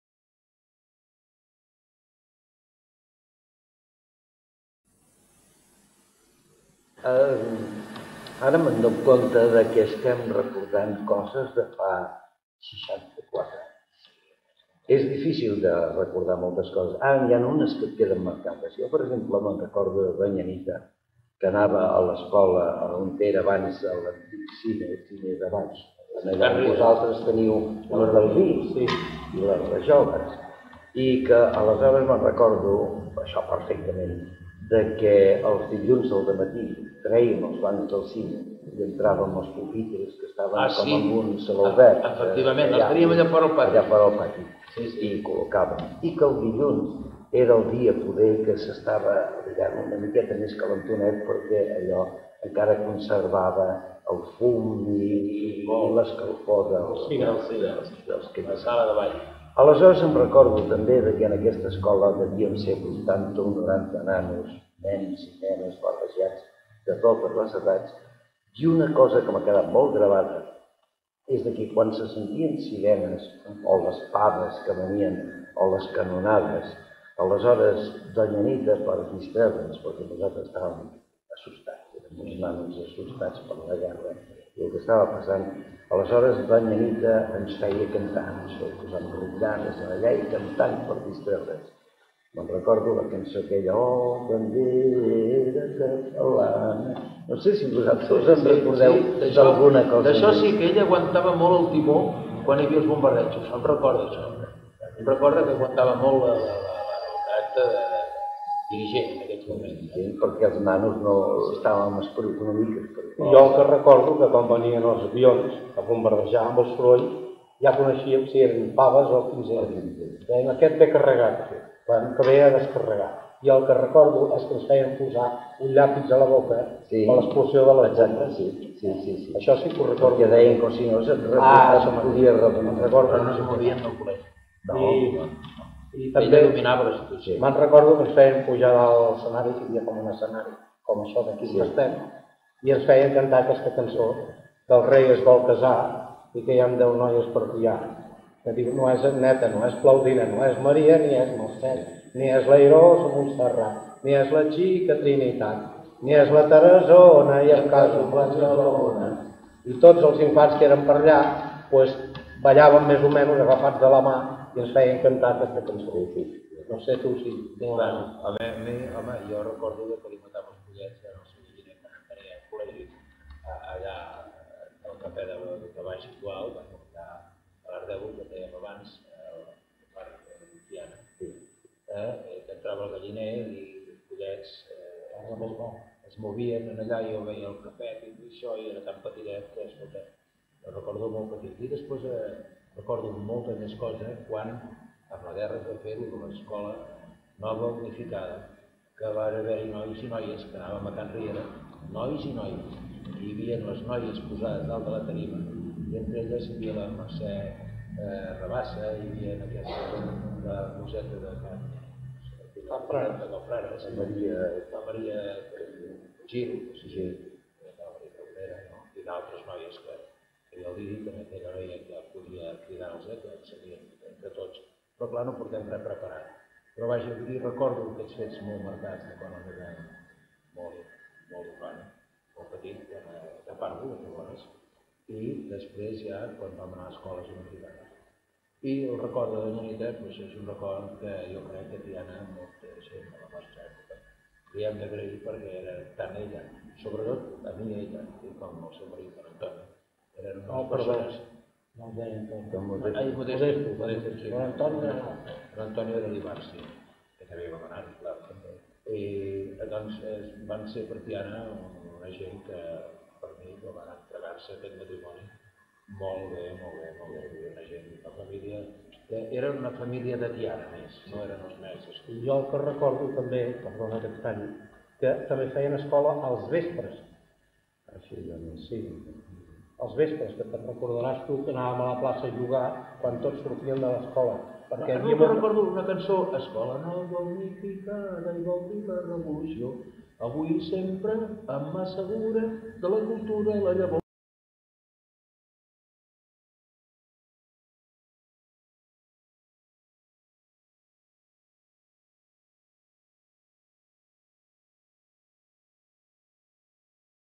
Doña Anita
Miquel i Joan Llongueras i Joan Pujol, alumnes de doña Anita, expliquen les classes a l'escola de Tiana durant la guerra civil.